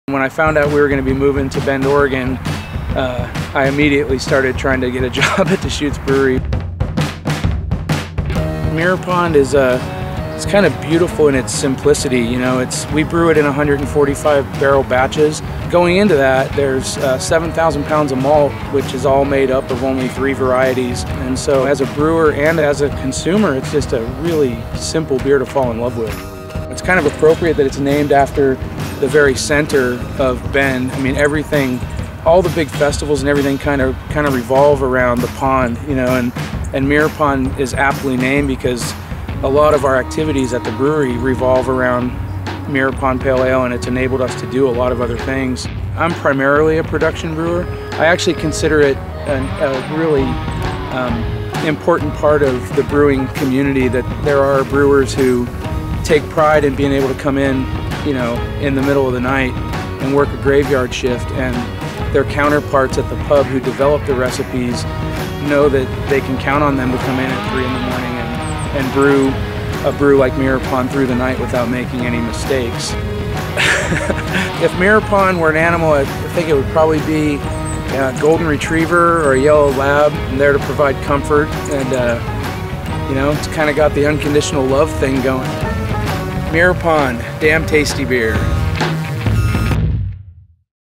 Brewer Film #3: Mirror Pond Pale Ale
Deschutes brewer John "Abe" Abraham talks about golden retrievers and unconditional love when it comes to Mirror Pond Pale Ale.